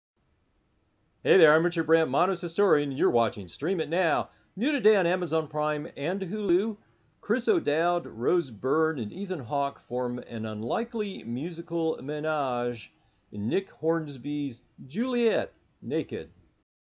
New on Amazon Prime and Hulu for June 24, 2019
The man of her dreams is Chris O'Dowd's worst nightmare in this quirky Nick Hornsby adaptation